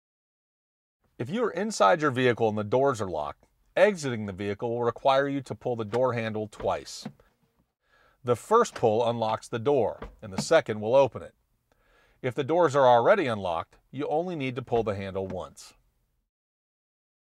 BMW Door Opening
In this video, learn how to exit a BMW vehicle using the door handle.